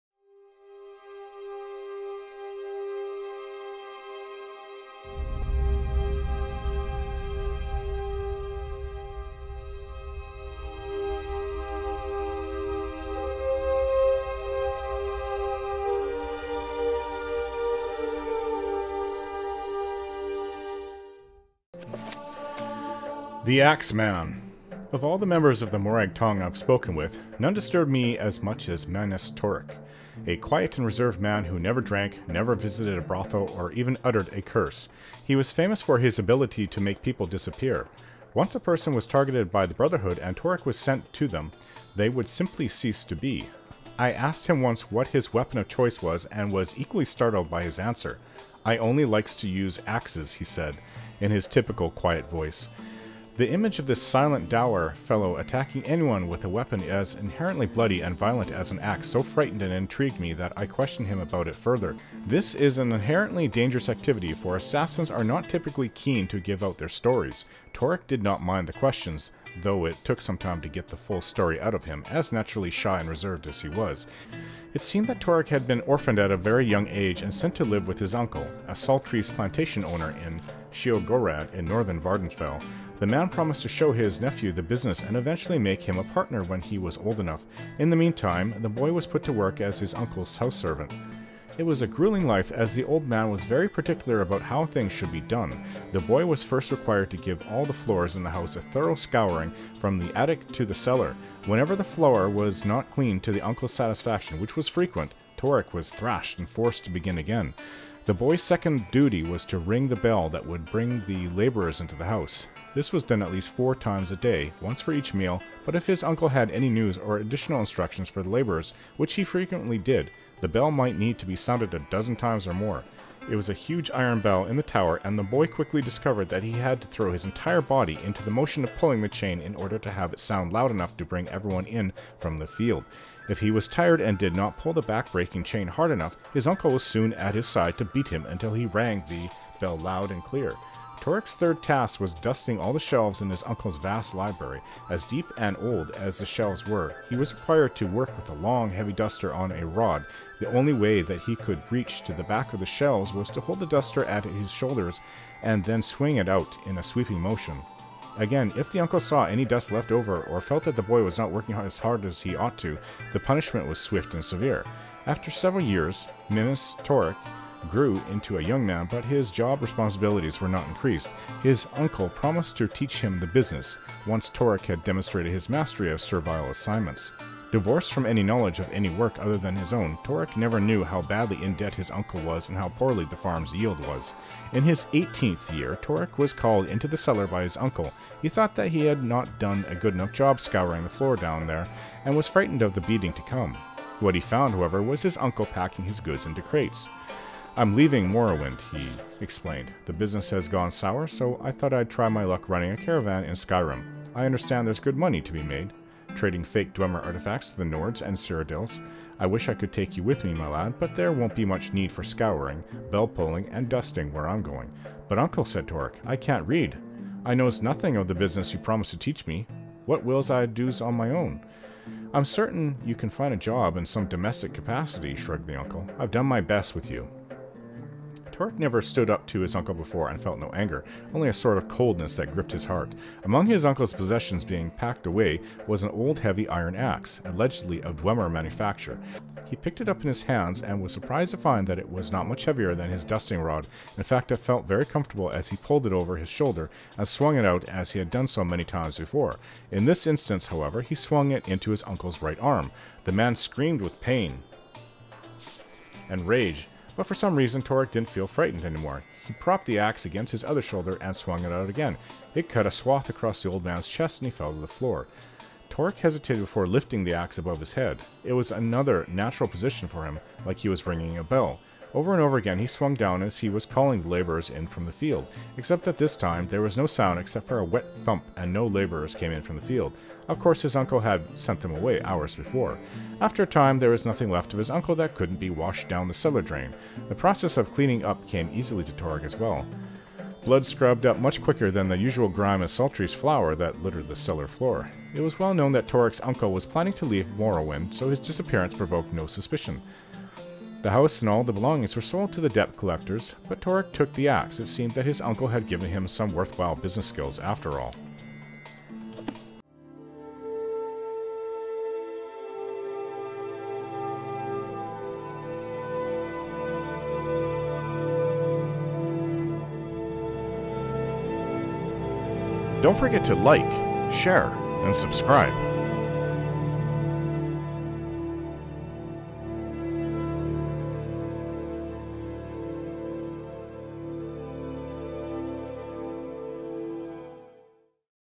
"The Axe Man"
The Axe Man cometh! No seriously, this is an extremely violent story of abuse, and then murdering your uncle because of it! Like all Elder Scrolls stories, don't just allow anyone to be in the room to listen to it. I wouldn't want my boys to hear it, it's way too violent!